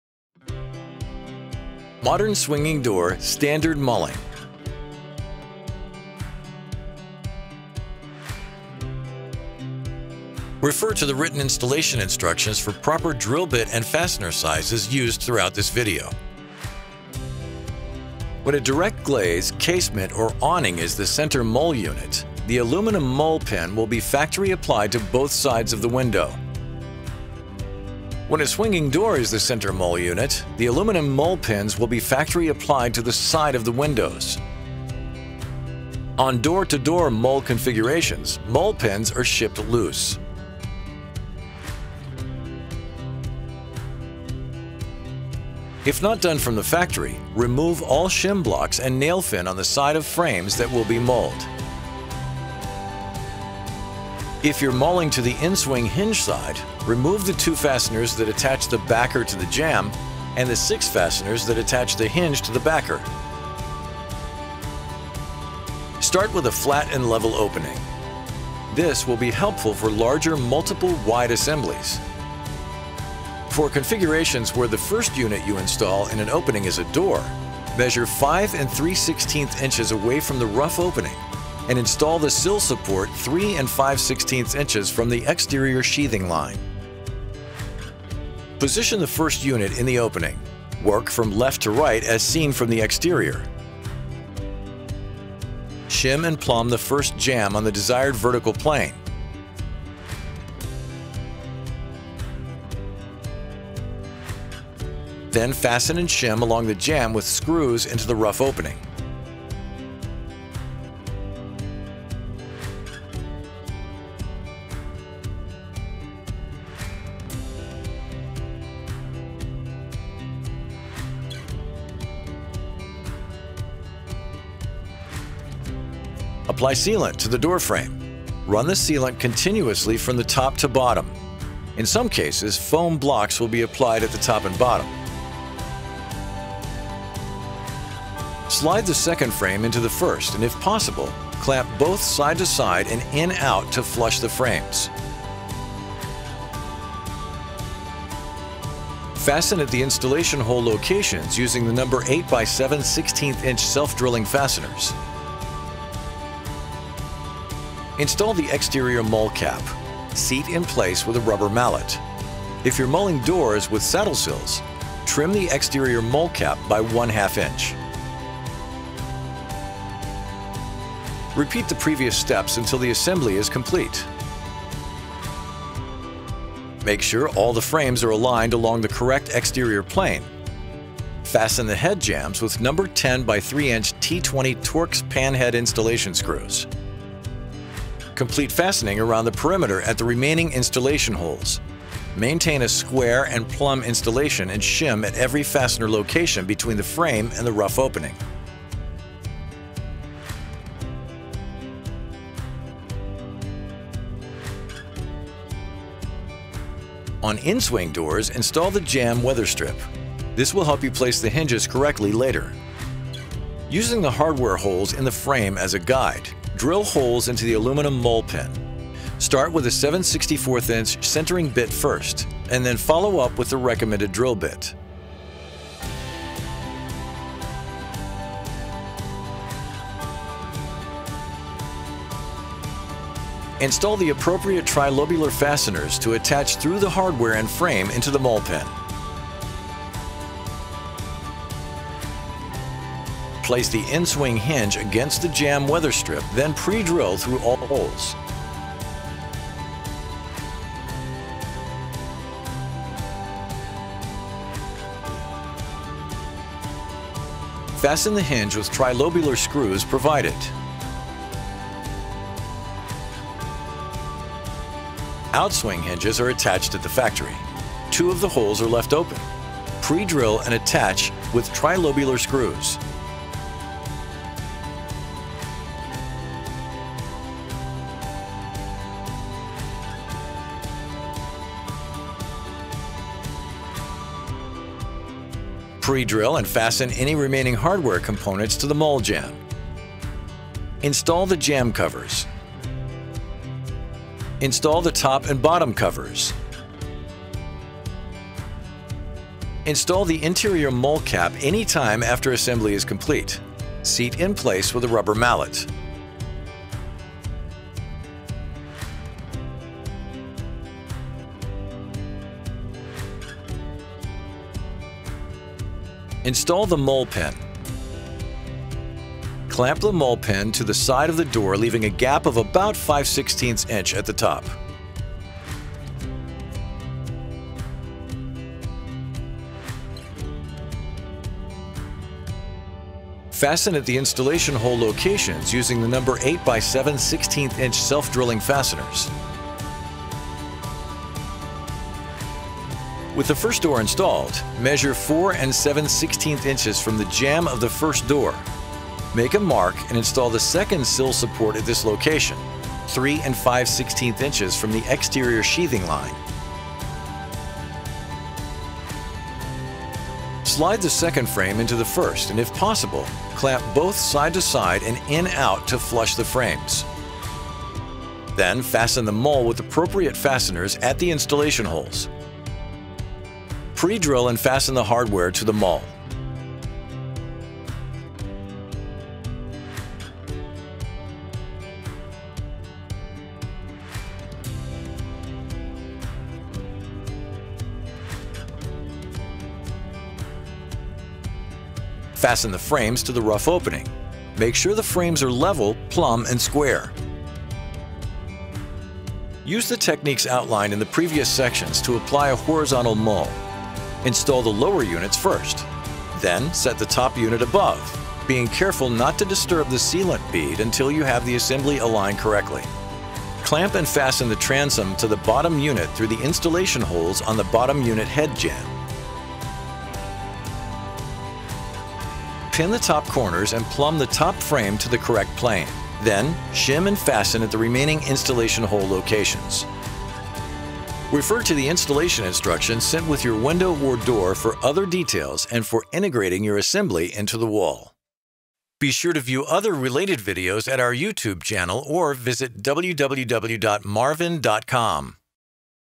Modern Swinging Door Standard Mulling
Animated procedure of Modern Swinging Door standard field mulls including door to window, door to door, and horizontal mulls.
TIMESTAMPS
00:00 Swinging Door Standard Mulling
00:43 Door to Window Mulling
01:09 Installing the Sill Support
05:11 Door to Door Mulling